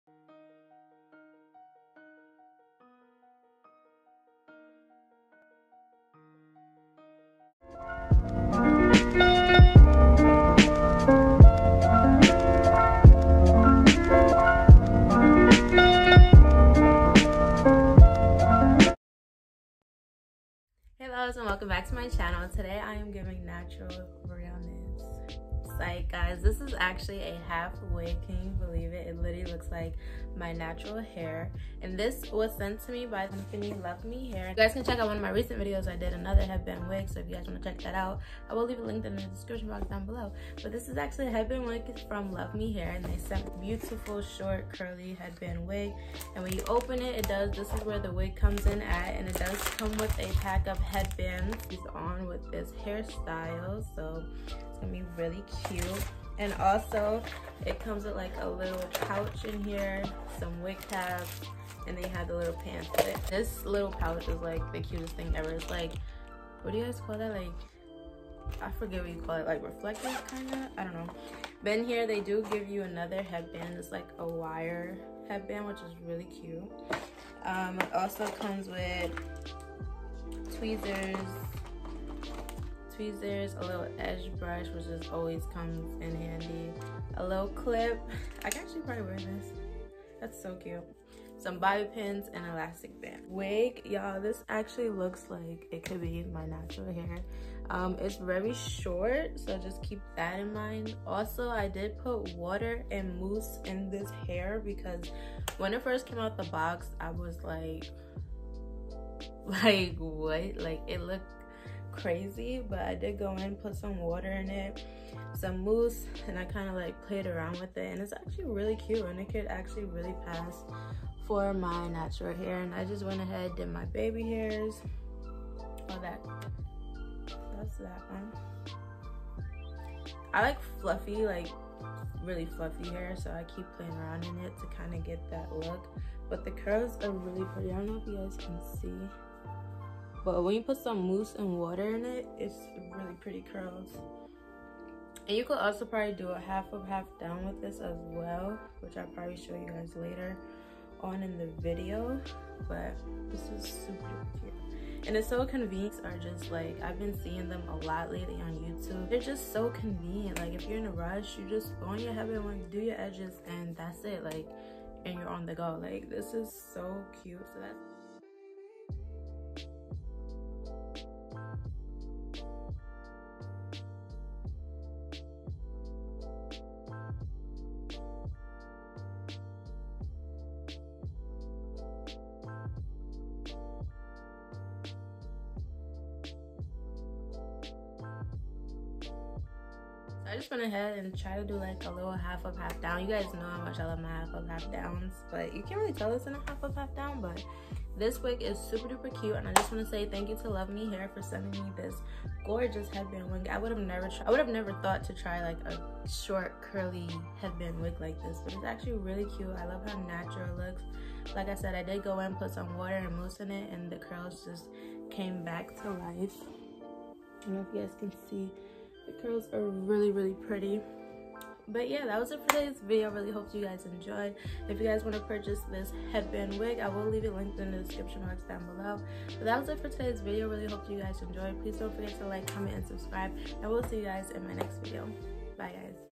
THE MOST NATURAL😍 LOOKING SHORT CURLY HEADBAND WIG💕 FT. LUVME HAIR
In today’s video I showed you all this beautiful short headband wig sent to me from luvmehair. This wig is so easy and convenient and perfect for those days your in a rush.

 ♡ Instagram ⇨ @llovealanna

 ♡ Twitter ⇨ @llovealanna

♡ Tik Tok ⇨ @lovelyalanna

HAIR INFO ↙️